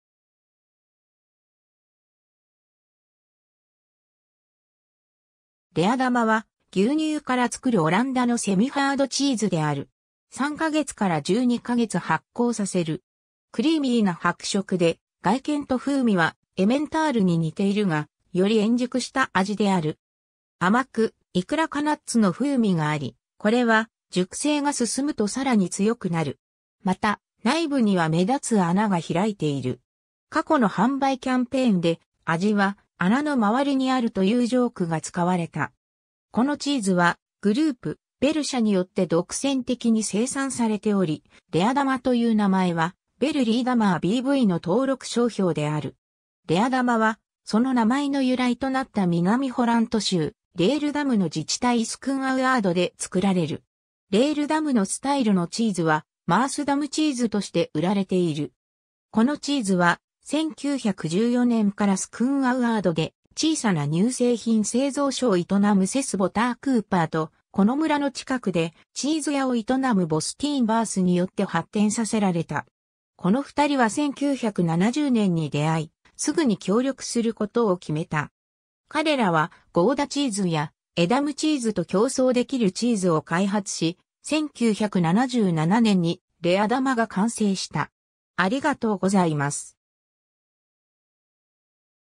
レアダマ
レアダマ(Leerdammer)は、牛乳から作るオランダのセミハードチーズである。
3ヵ月から12か月発酵させる。
クリーミーな白色で、外見と風味はエメンタールに似ているが、より円熟した味である。
甘く、いくらかナッツの風味があり、これは熟成が進むとさらに強くなる。
また、内部には目立つ穴が開いている。
過去の販売キャンペーンで、「味は穴の周りにある」というジョークが使われた。
 このチーズは、Groupe Bel社によって独占的に生産されており、レアダマという名前はBel Leerdammer B.V.の登録商標である。
 レアダマは、その名前の由来となった南ホラント州レールダムの自治体Schoonrewoerdで作られる。
レールダムのスタイルのチーズは、マースダムチーズとして売られている。
 このチーズは、1914年からSchoonrewoerdで小さな乳製品製造所を営むCees Boterkooperと、この村の近くでチーズ屋を営むBastiaan Baarsによって発展させられた。
この2人は1970年に出会い、すぐに協力することを決めた。
彼らは、ゴーダチーズやエダムチーズと競争できるチーズを開発し、1977年にレアダマが完成した。